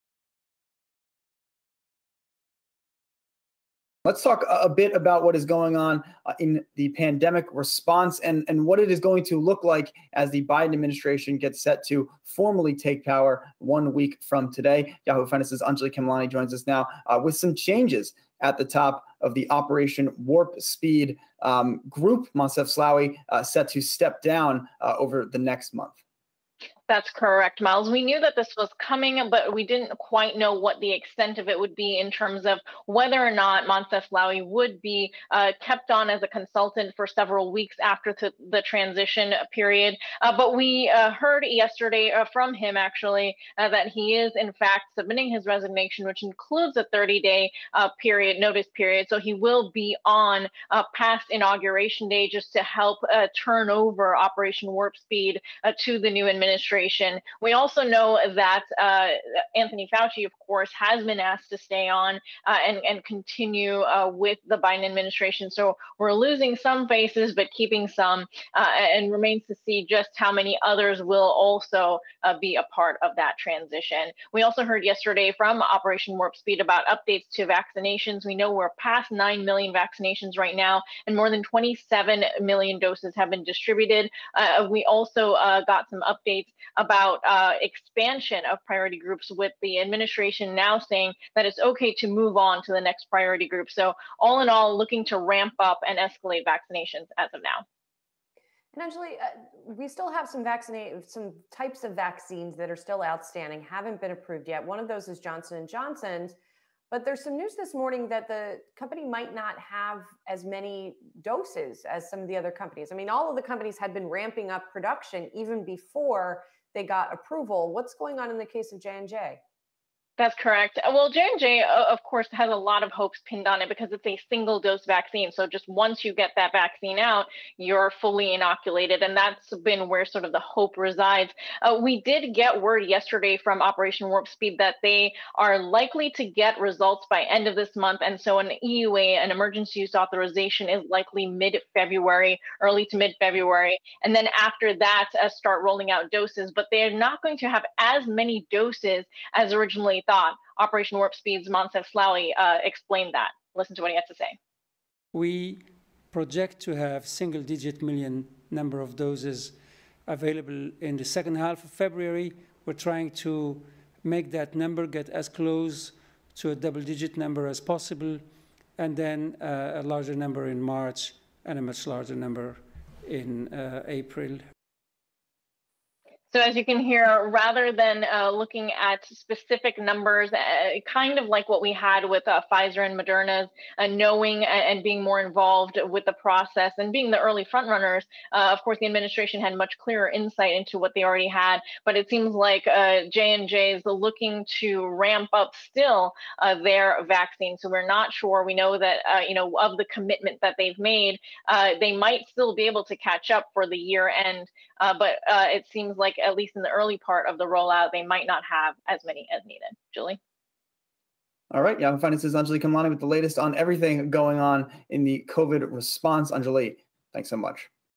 Operation Warp Speed Chief chief Monclef Slaoui resigns, Dr. Fauci will stay on
Yahoo Finance's Anjalee Khemlani reports the latest news on Operation Warp Speed Chief Monclef Slaoui resigning but remaining through Biden transition and Dr. Anthony Fauci staying on.
Also, Anjalee reports on the latest efforts to ramp up vaccine distribution.